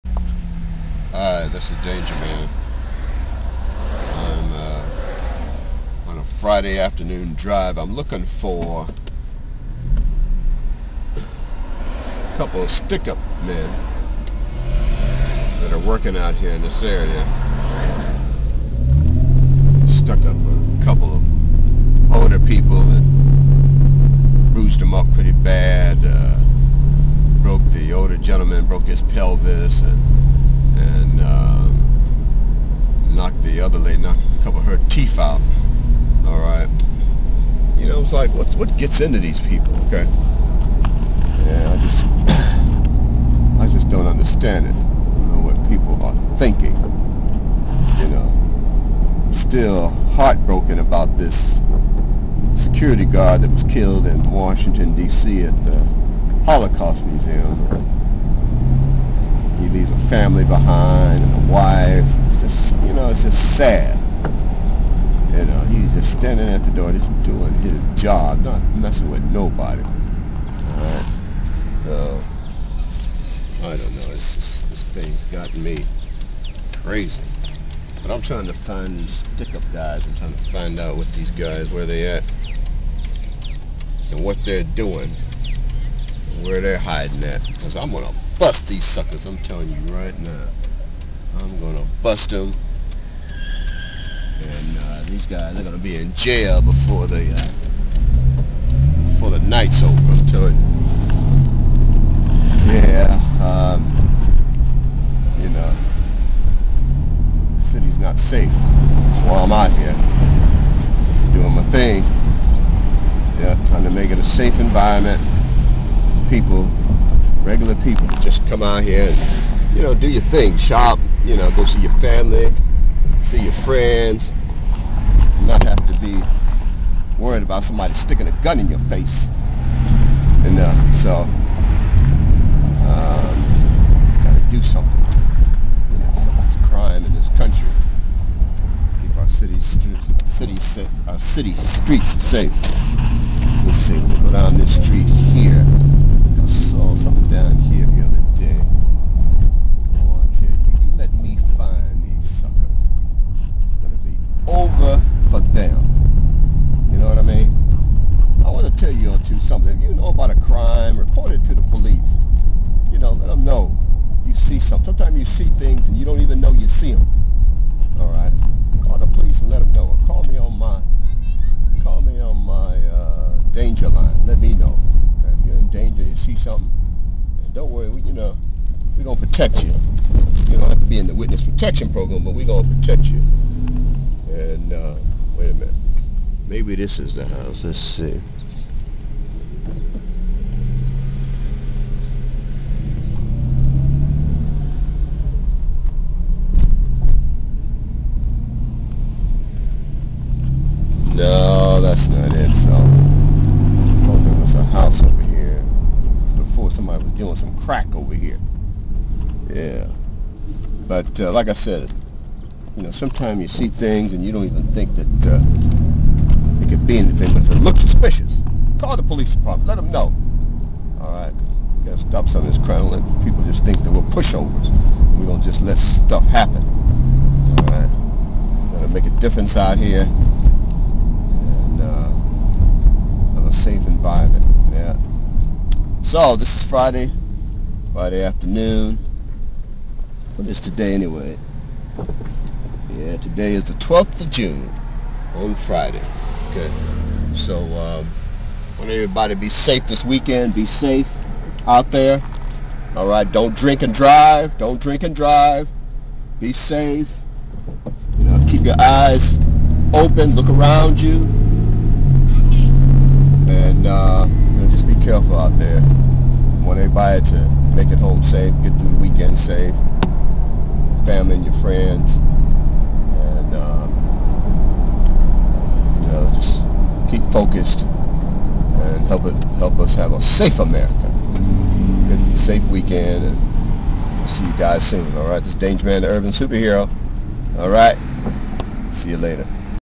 DANGERMAN ASKS "Where the Stick-Up Boys At?"
DANGERMAN is at it again, I'm searching for the crew dubbed the West Side Stick-Up Boys" they have been terrorizing the city for to long. I will not rest until they are behind bars. Be safe out there, just dial  D A N G E R  on your cell if you need me.

DANGERMAN